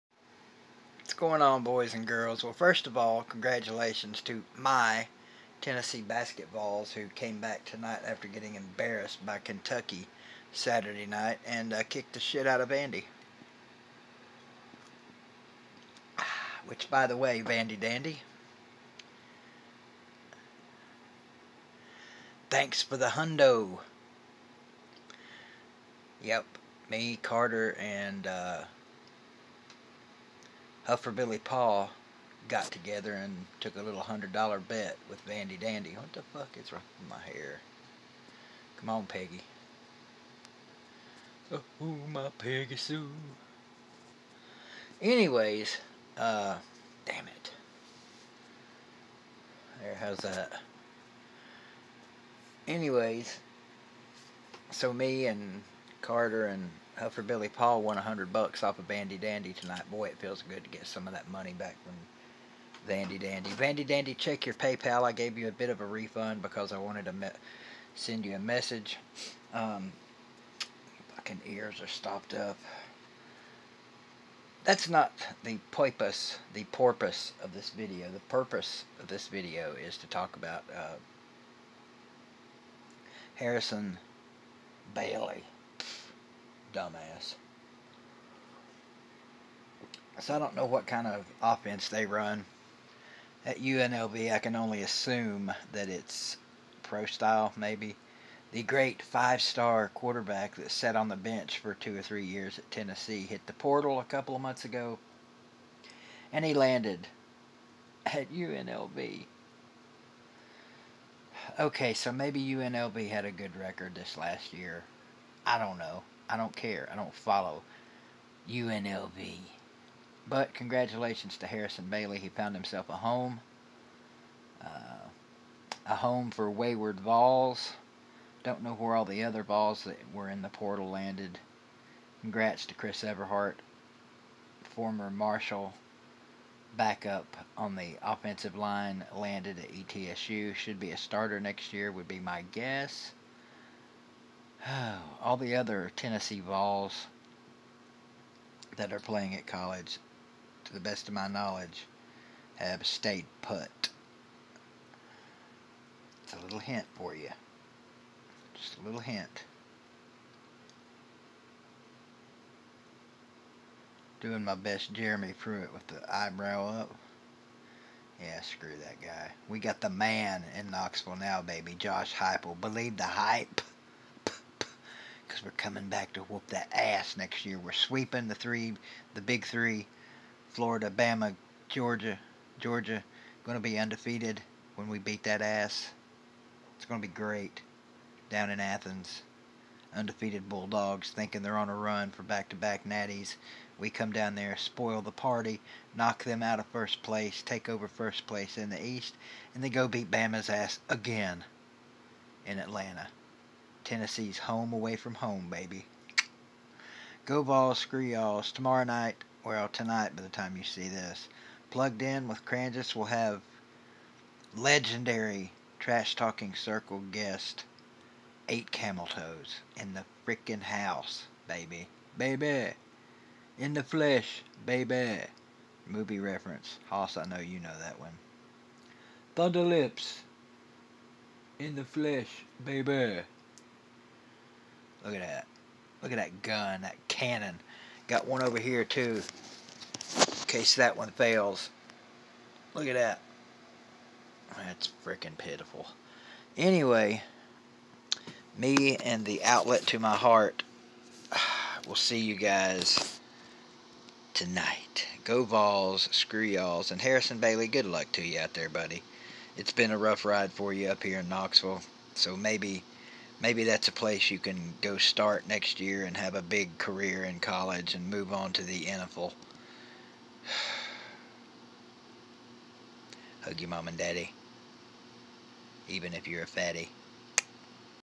Harrison Bailey finds a home he can GAMBLE with
former VOL Harrison Bailey has landed in the desert. Could this be the home he was looking for all along? Time will tell. Good luck young man and NO unlv did not have a good record unless 2-10 is good for them.